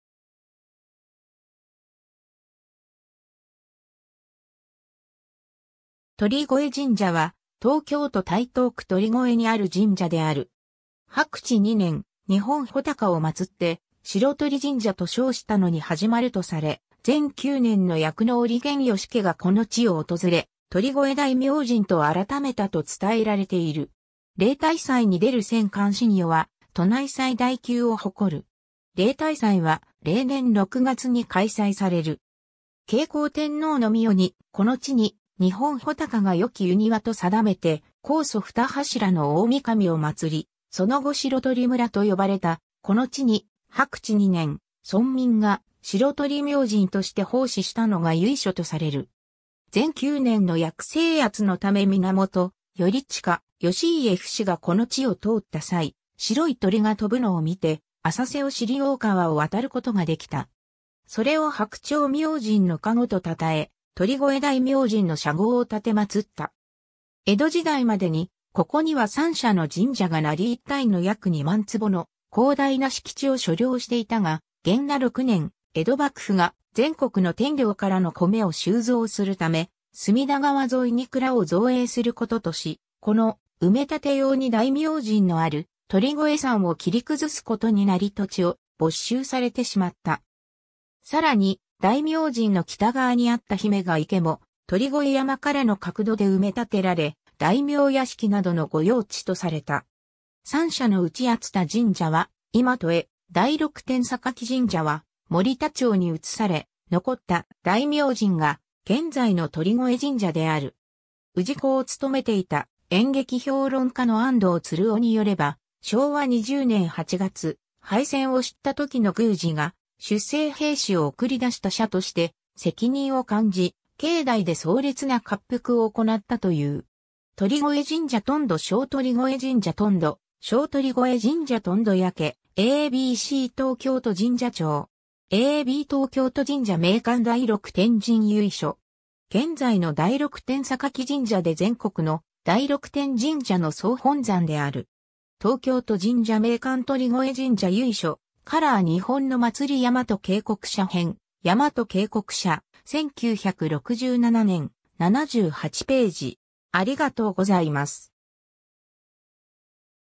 鳥越神社
鳥越神社（とりこえじんじゃ）は、東京都台東区鳥越にある神社である。
白雉2年（651年）、日本武尊を祀って白鳥神社と称したのに始まるとされ、前九年の役のおり源義家がこの地を訪れ鳥越大明神と改めたと伝えられている。
例大祭に出る千貫神輿は都内最大級を誇る。
 例大祭（鳥越祭、鳥越まつり）は、例年6月に開催される。
 景行天皇の御代に、この地に日本武尊が良き斎庭と定めて皇祖二柱の大御神を祀り、その後白鳥村とよばれたこの地に白雉2年（651年）、村民が「白鳥明神」として奉祀したのが由緒とされる。
 前九年の役征圧のため源頼義、義家父子がこの地を通った際、白い鳥が飛ぶのを見て浅瀬を知り大川（隅田川）を渡ることができた。
それを白鳥明神の加護とたたえ、鳥越大明神の社号を奉った。
 江戸時代までにここには三社の神社が成り一帯の約2万坪の広大な敷地を所領していたが、元和6年（1620年）、江戸幕府が全国の天領からの米を収蔵するため、隅田川沿いに蔵（浅草御蔵）を造営することとし、この埋め立て用に大明神のある鳥越山を切り崩すことになり土地を没収されてしまった。
さらに、大明神の北側にあった姫ヶ池も鳥越山からの客土で埋め立てられ、大名屋敷などの御用地とされた。
 三社のうち熱田神社は今戸へ、第六天榊神社 は森田町（現・蔵前3丁目）に遷され、残った大明神が現在の鳥越神社である。
 氏子をつとめていた演劇評論家の安藤鶴夫によれば、昭和20年8月、敗戦を知った時の宮司が、出征兵士を送り出した社として責任を感じ、境内で壮烈な割腹をおこなったという。
 鳥越神社とんど焼 鳥越神社とんど焼 鳥越神社とんど焼 ^ a b c 東京都神社庁. ^ a b 東京都神社名鑑 第六天神 由緒。
現在の第六天榊神社で全国の第六天神社の総本山である。
 ^ 東京都神社名鑑 鳥越神社 由緒 ^ 『カラー 日本のまつり』山と渓谷社編、山と渓谷社、1967年、78頁。